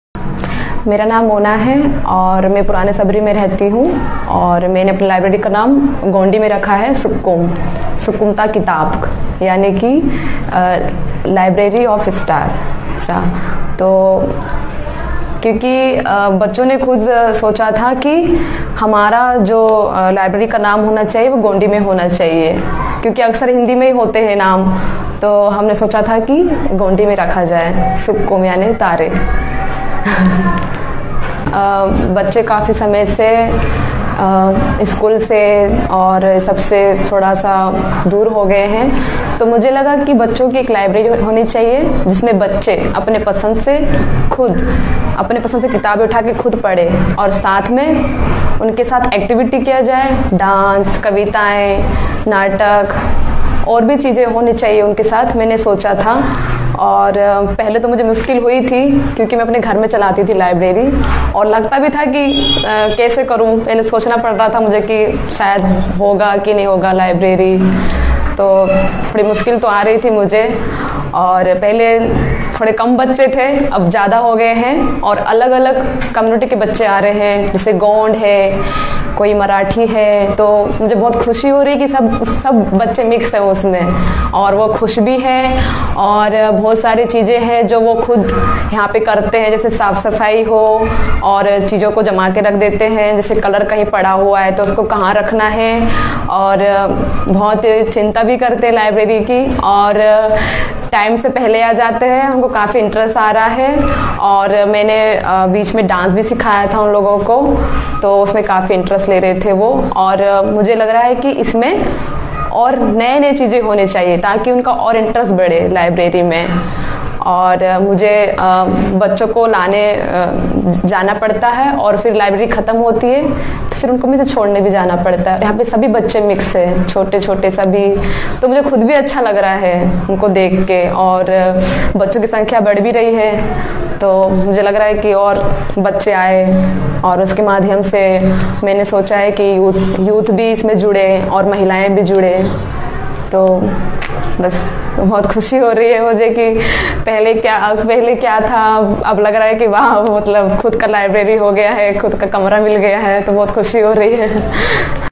अंतर्राष्ट्रीय एकजुटता IWA , मुक्तिवादी एकता मोर्चा (उदारवादी एकता के मोर्चा) के हिंदू वर्ग के साथी
Sukkum ta Kitaapk (लाइब्रेरी ऑफ़ स्टार्स) के सह-लाइब्रेरियन और MEM Muktivadi एकता मोर्चा में UVT के सदस्य, मोना भोपाल में शबरी नगर में लाइब्रेरी स्थापित करने के पीछे उनकी प्रेरणाओं और प्रेरणा के बारे में बात करते हैं।  हम वर्तमान में पुस्तकालय को आपूर्ति और फर्नीचर के साथ सुसज्जित करने के लिए नए साल से पहले 40 वें वर्ष का लक्ष्य बना रहे हैं, साथ ही आवश्यक वस्तुओं की खरीद और किराए का भुगतान भी करते हैं।  उनके पहले के पदों और पुस्तकालय के बारे में अधिक जानकारी प्राप्त करें!

 अंतर्राष्ट्रीय एकजुटता का आह्वान महामारी की शुरुआत में, AIT के हिंदू वर्ग के साथियों, मुक्तिवादी एकता मोर्चा (लिबर्टीयन यूनिटी का मोर्चा), ने द लाइब्रेरी ऑफ़ द स्टार्स (सुक्कुम ता कीतापक) नामक एक स्कूल खोलने का फैसला किया, जहाँ उन्होंने बेटियों का स्वागत किया।  और कमजोर परिस्थितियों में श्रमिकों के बच्चे।  उन्होंने हाल ही में परियोजना के बारे में सूचित करने के लिए एक अंतर्राष्ट्रीय अभियान शुरू किया है, जिसमें फर्नीचर, स्कूल की कंडीशनिंग, और पुस्तकों और स्कूल की आपूर्ति के अधिग्रहण के लिए धन जुटाने के लिए।  अगर आप मदद करना चाहते हैं या आपके पास पैसे बचे हैं और आप नहीं जानते कि इसका क्या करना है।  यहाँ आप बहुत मदद कर सकते हैं:

 स्थानांतरण विवरण:

 नाम: मिस मोना ए / सी नं .: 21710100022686 IFSC कोड: BARBOKOTRAS Google वेतन: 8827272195

 कॉमरेड मोना, सुक्कम टा कितापक स्कूल के सह-संस्थापक और एमईआर-एआईटी इंडिया के अनारचो-सिंडिकलिस्ट आतंकवादी, उन चिंताओं और प्रेरणाओं की व्याख्या करते हुए, जिन्होंने उन्हें इस अनमोल परियोजना को शुरू करने के लिए प्रेरित किया।
Co-librarian of Sukkum ta Kitaapk (Library of Stars) and member of the UVT at MEM Muktivadi Ekta Morcha, Mona talks about her motivations and inspiration behind setting up the library at Shabri Nagar in Bhopal. We are currently aiming yo fundraise 40k before the new year to equip the library with supplies and furniture, as well as procure essentials and pay rent. Find details in their earlier posts and more about the library!

Llamada a la Solidaridad internacional Las compañeras de la sección hindú de la AIT, Muktivadi Ekta Morcha (Frente de Unidad Libertaria), decidieron abrir a comienzos de la pandemia una escuela llamada La Librería de las Estrellas (Sukkum ta Kitaapk) donde acogieron a las hijas e hijos de trabajadoras en situaciones de vulnerabilidad. Recientemente han comenzado una campaña internacional informando del proyecto, con el fin de recaudar fondos destinados al mobiliario, el acondicionamiento de la escuela, y la adquisición de libros y material escolar. Si quieres echar una mano o te sobra dinero y no sabes qué hacer con él. Aquí puedes ser de gran ayuda:

Transfer details:

Name: Miss Mona A/C No.: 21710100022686 IFSC Code: BARBOKOTRAS Google Pay: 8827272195

La compañera Mona, cofundadora de la escuela Sukkum ta Kitaapk y militante anarcosindicalista de MEM-AIT India, explicando las inquietudes y motivaciones que les llevaron a embarcarse en este precioso proyecto.